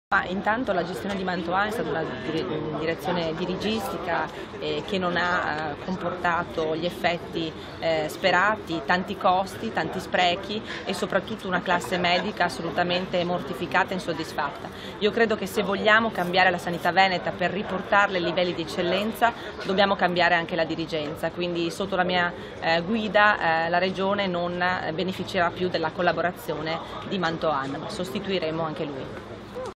SANITA' VENETA DIRIGENZA DA CAMBIARE
Per riportare la Sanità del Veneto al livelli che le competono c'è la necessità di rinnovare la dirigenza. La candida... [continua a leggere su